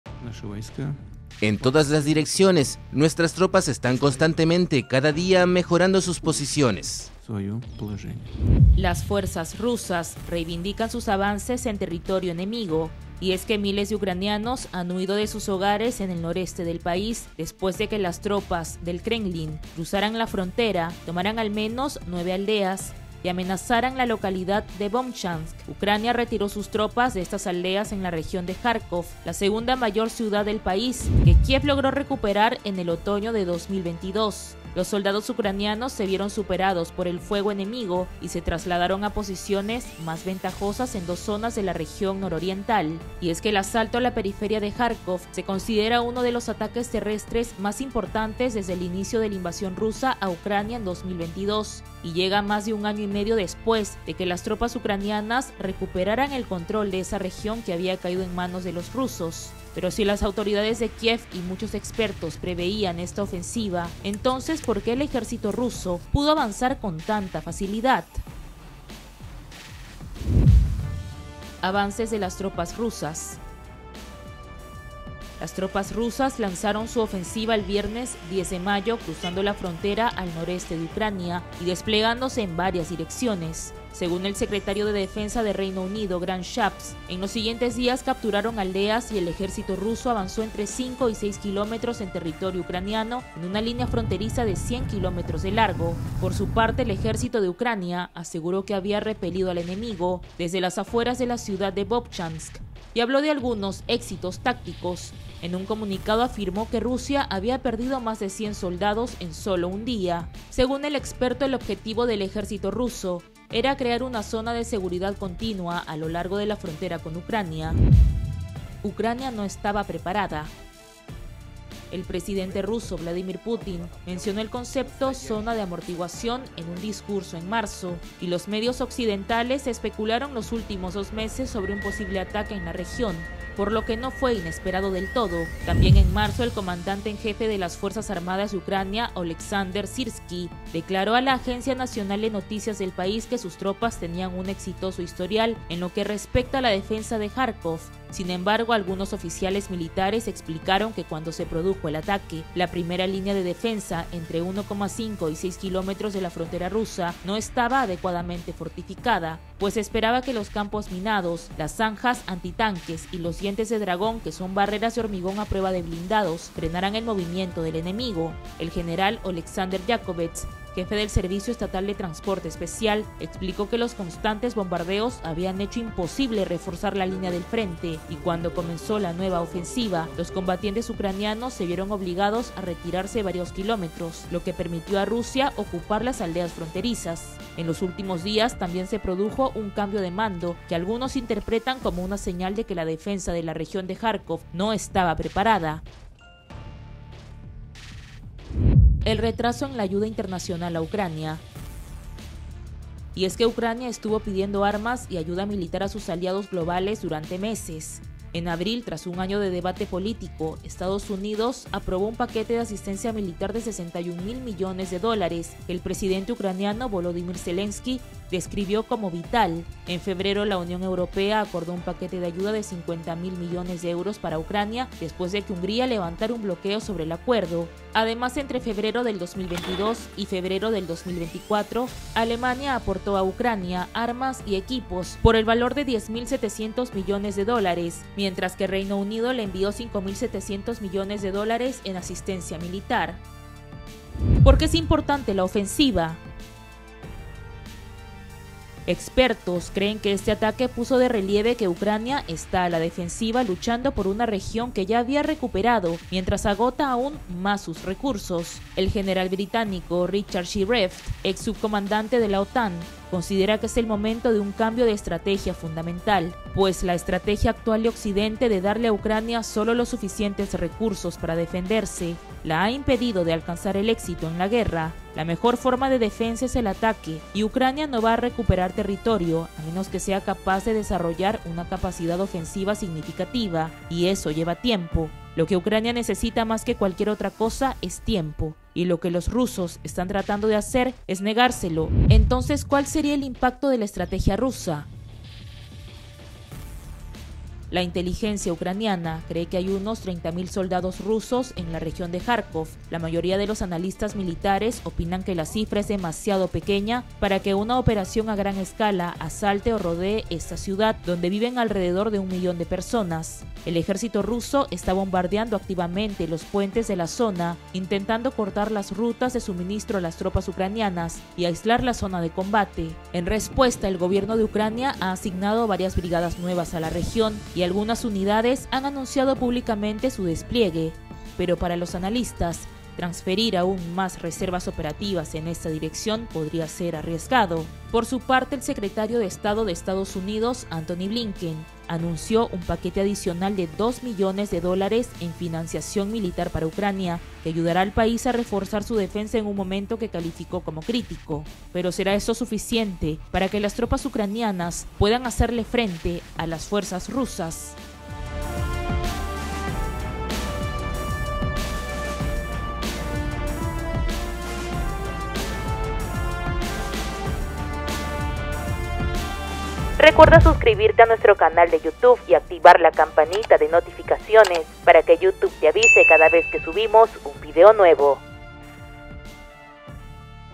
GRAN OFENSIVA RUSA EN JARKOV: 5 claves para entender su avance el NORTE Y SUR de UCRANIA
Miles de ucranianos han huido de sus hogares en el noreste del país después de que las tropas rusas cruzaran la frontera, tomaran al menos nueve aldeas y amenazaran la localidad de Vovchansk.

Ucrania retiró sus tropas de estas aldeas en la región de Járkov, la segunda mayor ciudad del país que Kiev logró recuperar en el otoño de 2022.

Los soldados ucranianos se vieron superados por el fuego enemigo y se trasladaron a "posiciones más ventajosas" en dos zonas de la región nororiental, afirmó un portavoz militar, lo que indica una retirada.